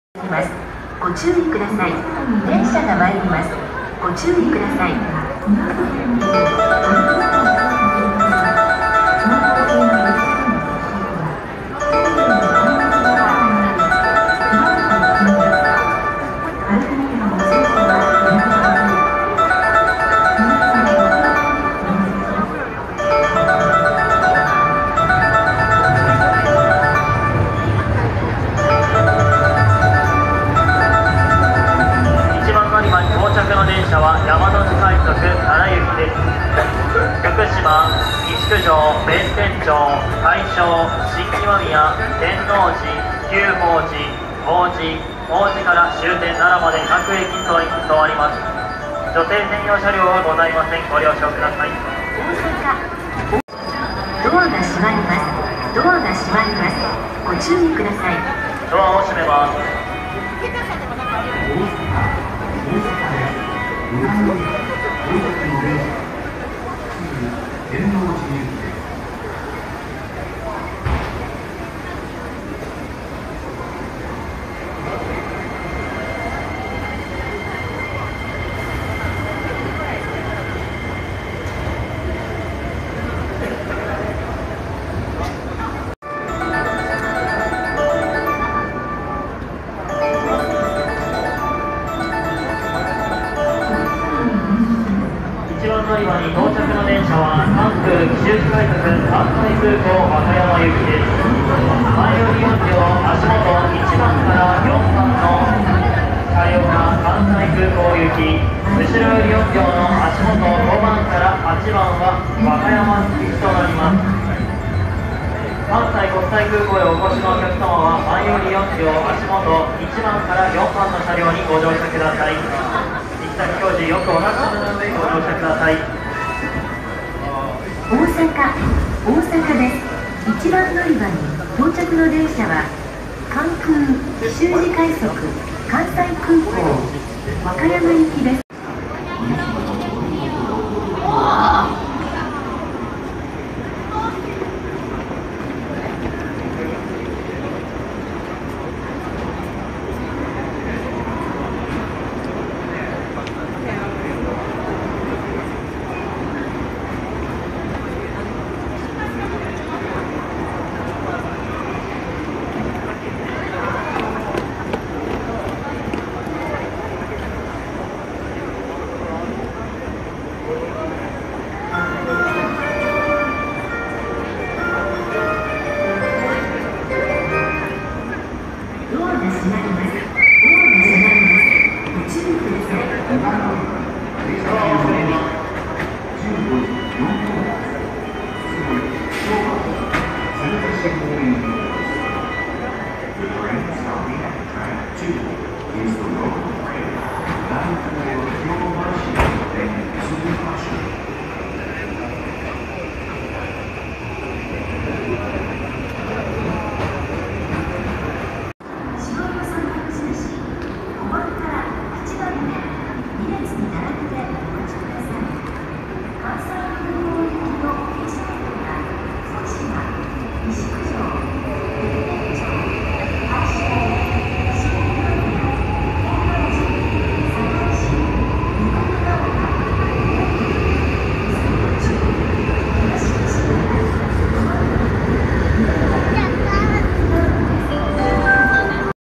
大阪環状線にもホームドア設置完了 大阪駅1番のりば ホームドア稼働の様子
京橋駅に続いて、大阪駅の一番乗り場にもホームドアが設置されました。(外回りホームは、まだ設置されていません。)今回はその様子を撮影しました。ぜひ最後までご覧下さい。

2020年JR西日本ダイヤ改正